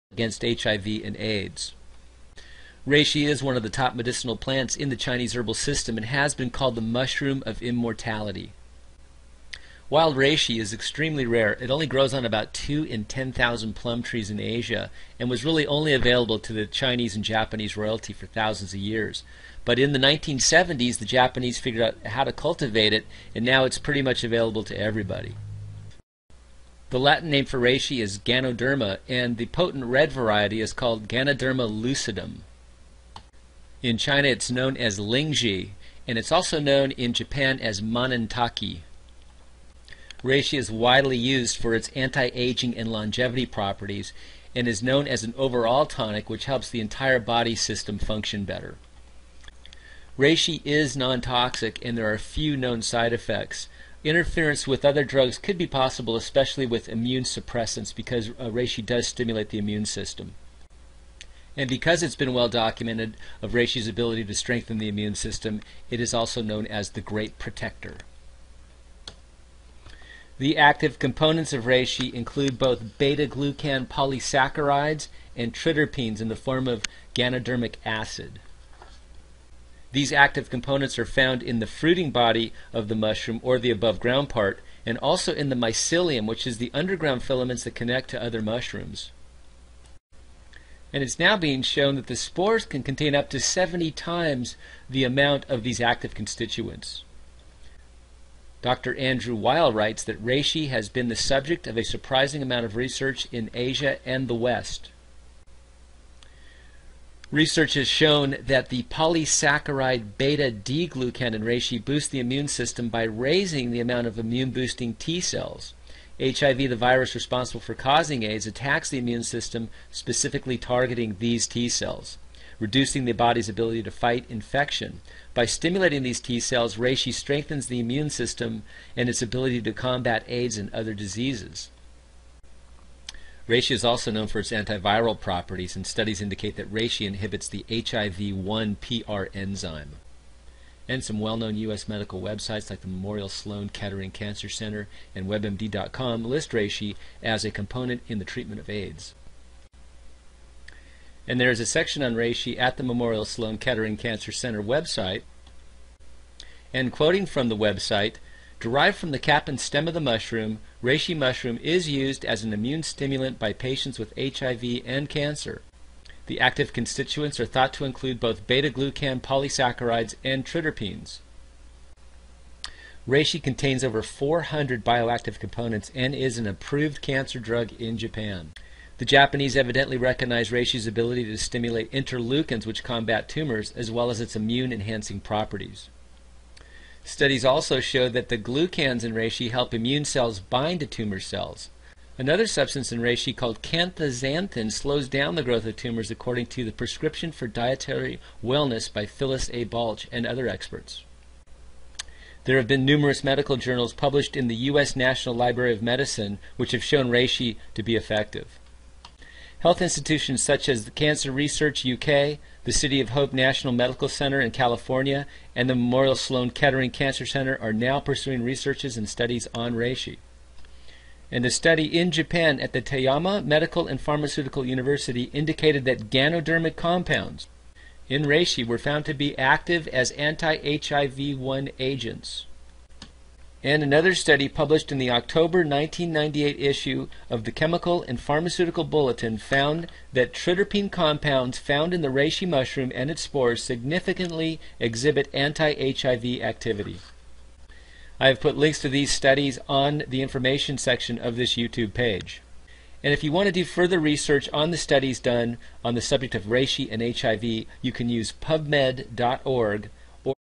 Reishi | Ganoderma | HIV and Cancer remarkable results
Reishi or Ganoderma Lucidum Clinical Resuts on HIV and Cancer
as reported around the World from Japan to USA